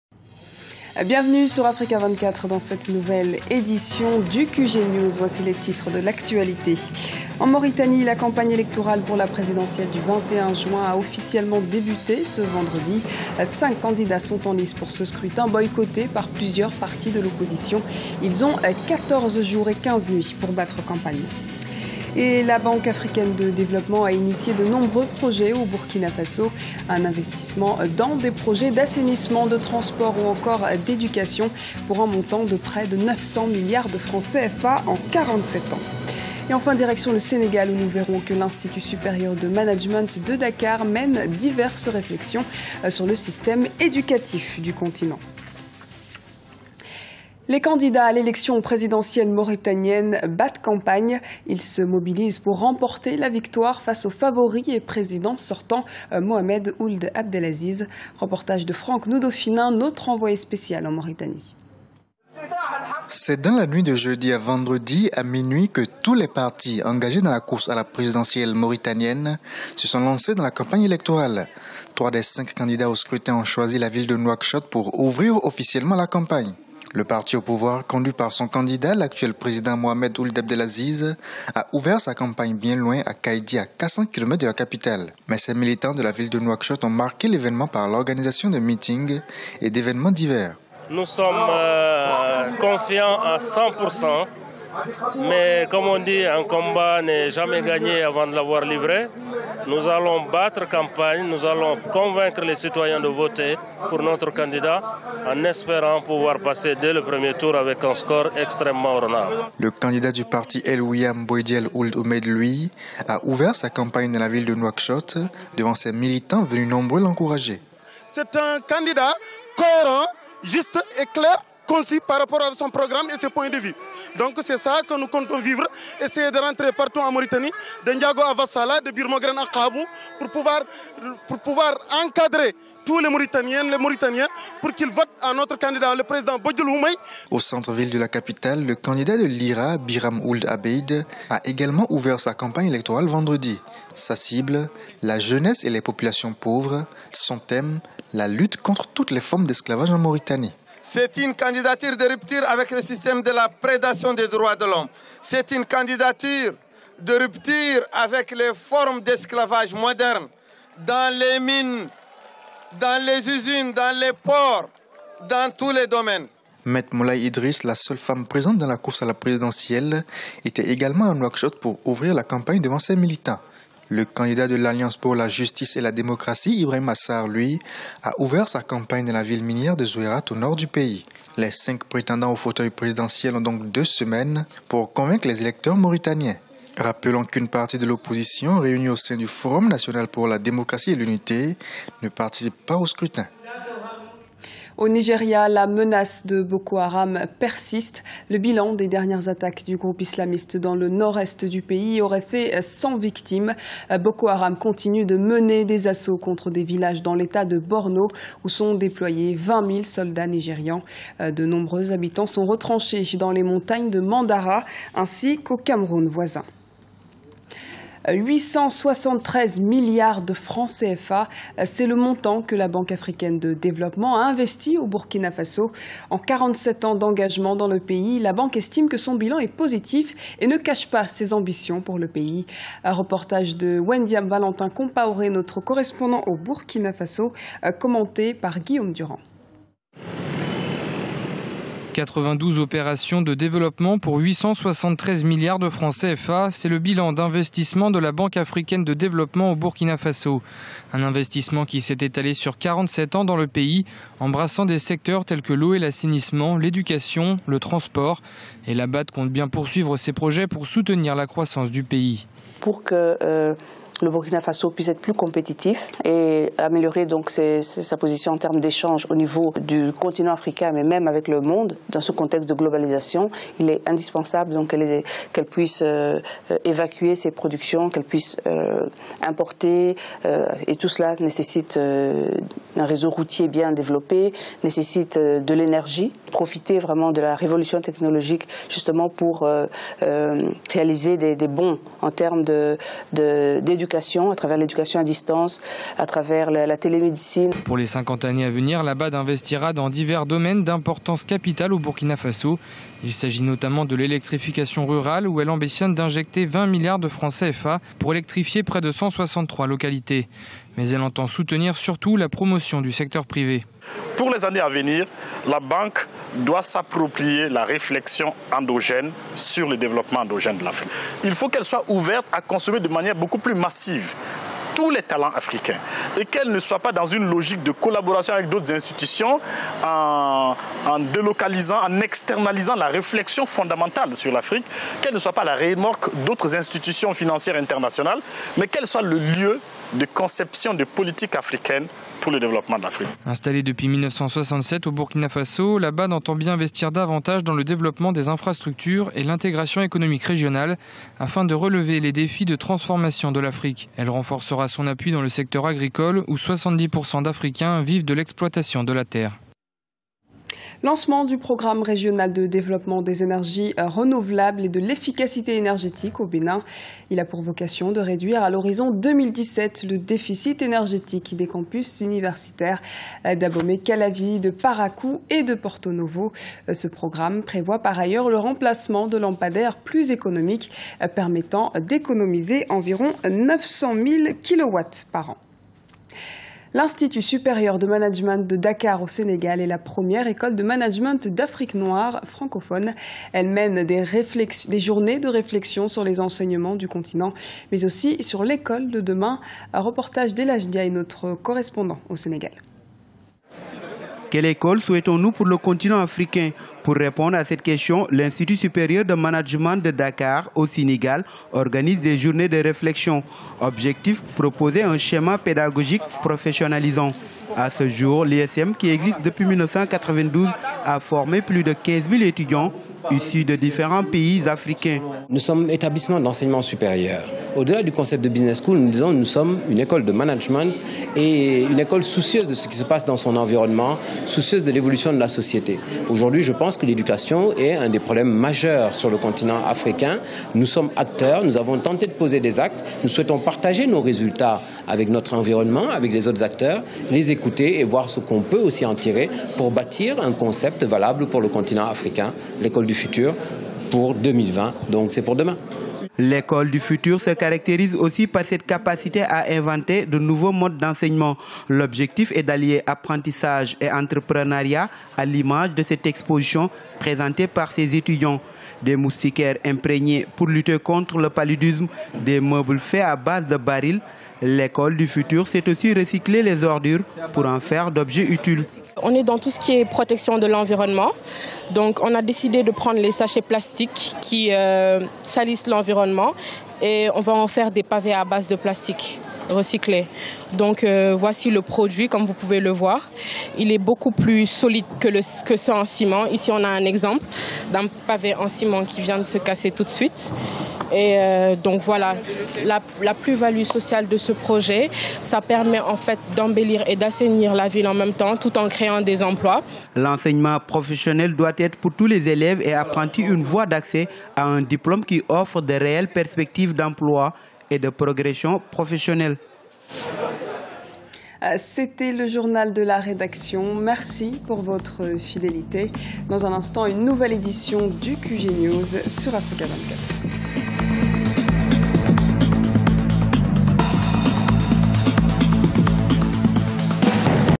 Le Journal du vendredi 6 juin - 15h GMT
Mauritanie, Début de la campagne présidentielle.
Burkina Faso, La BAD investit dans de nombreux secteurs. Sénégal, Le groupe ISM à l'avant-garde de la formation.